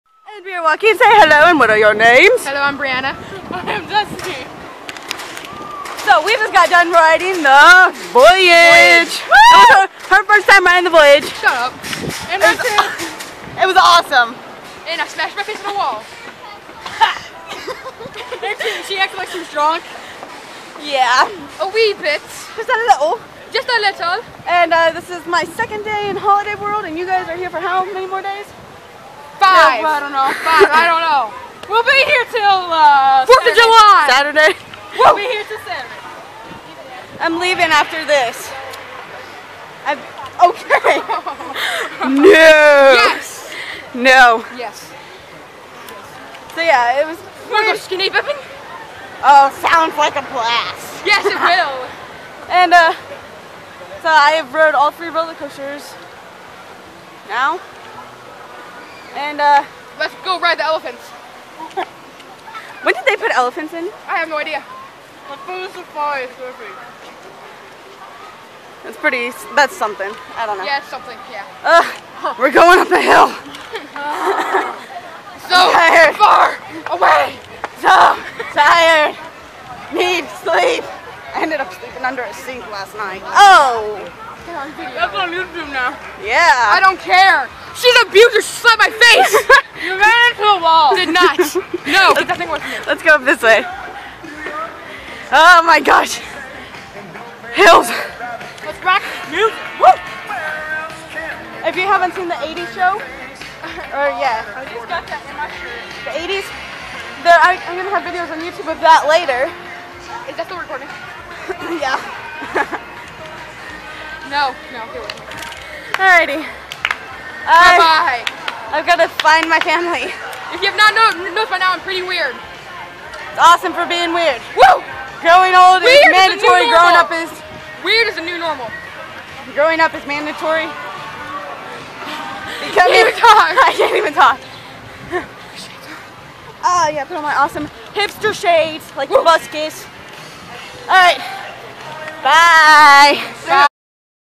Day 2: The Voyage
I met some new friends, Breanna and her sister Destiny. And it was their first time on the Voyage.